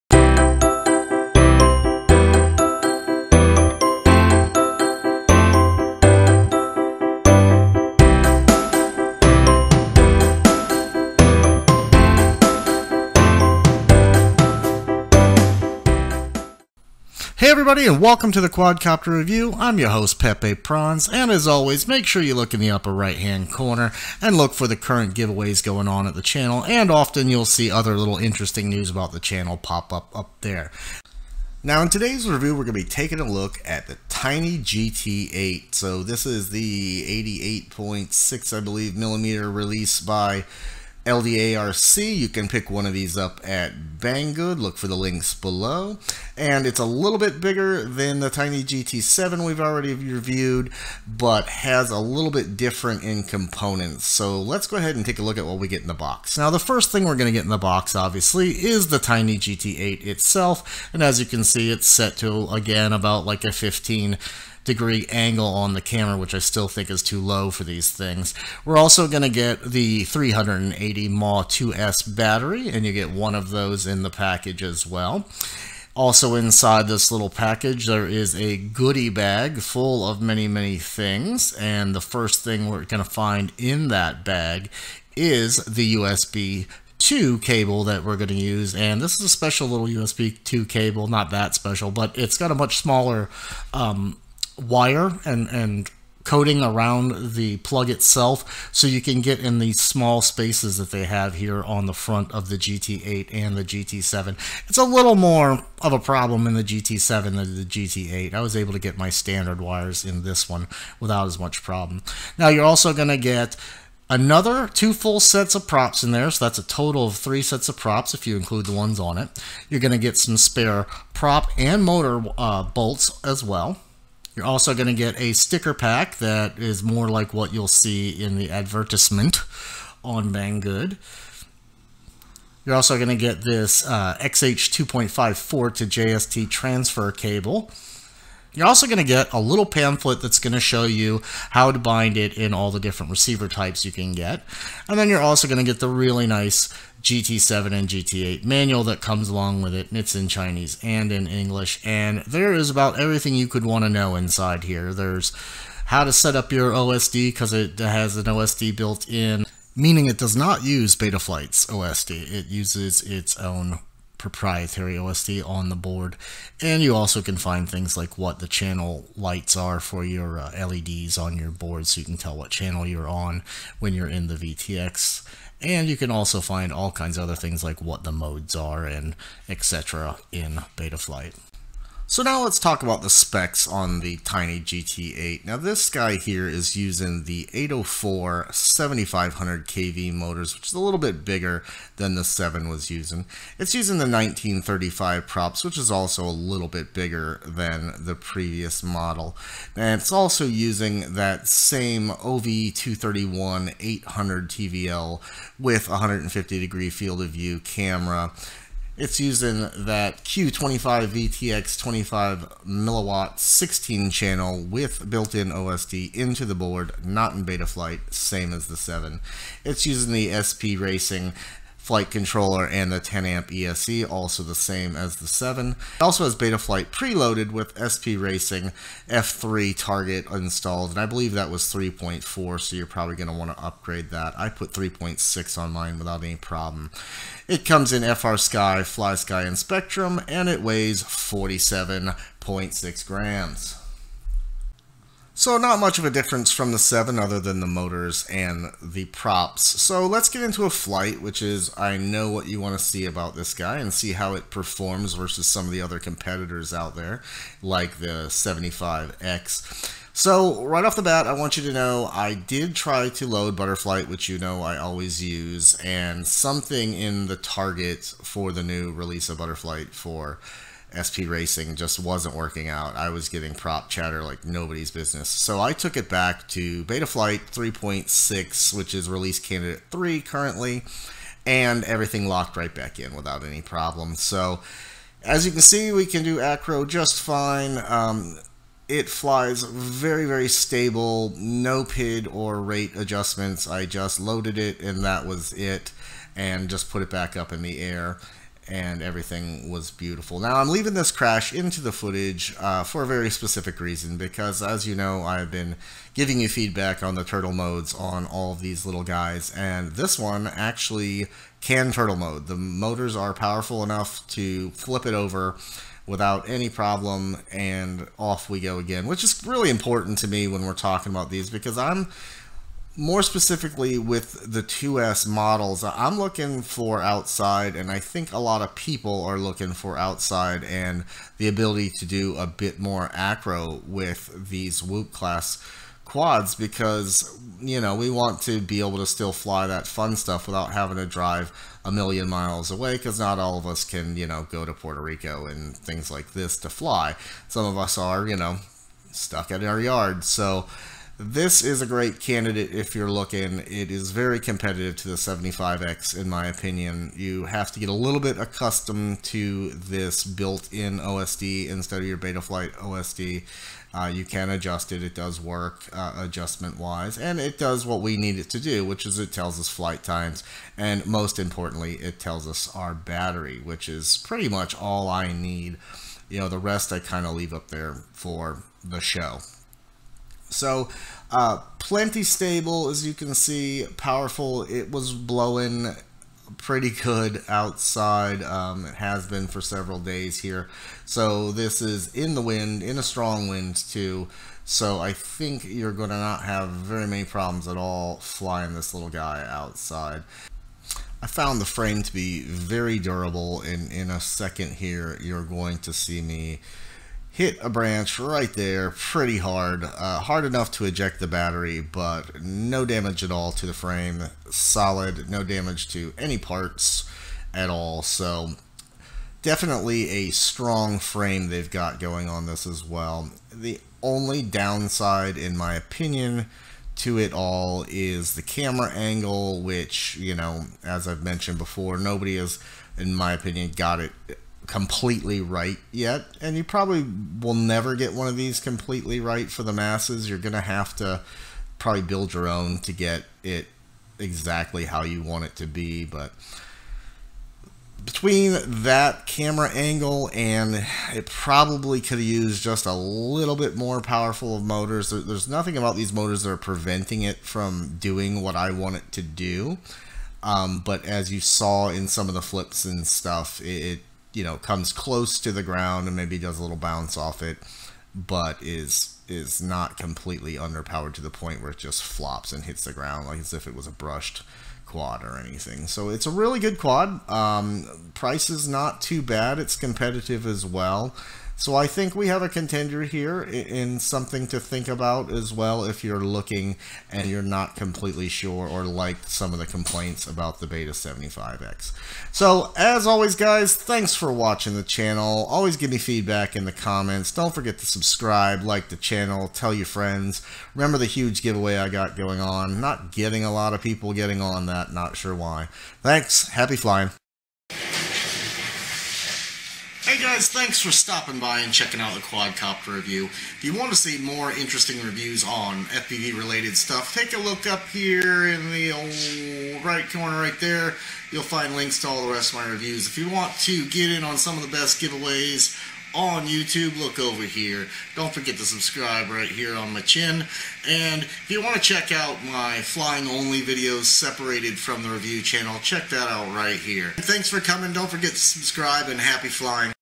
King Kong / LDARC Tiny GT8 - Unboxing, Review & Test Flight
Click SHOW MORE to see the links mentioned in this video.

The Humming Store
BetaFPV Store

Today we're taking a look at the Tiny GT7's big brother the TINY GT8 87.6mm FPV Racing Drone lets talk specs.
Specification:
Frame wheelbase: 87.6mm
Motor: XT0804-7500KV
Propeller: 1935
Flight Controll+ESC: Betaflight SPRCINGF3 + 4 in 1 10A Blheli_S ESC
Receiver: RX2A PRO(Flysky), DSM2(Spektrum), D16(Frsky Non-EU)
Camera: OV231(800TVL 150 degreee NTSC)
VTX: Q25 OSD (25mW 16CH Smart OSD V1)
Input Voltage: Only 2S
Battery: 7.4V 380mAh 50C
Weight: 47.8g (no receiver and battery)
Package weight: 451.4g
Package size: 214*184*90mm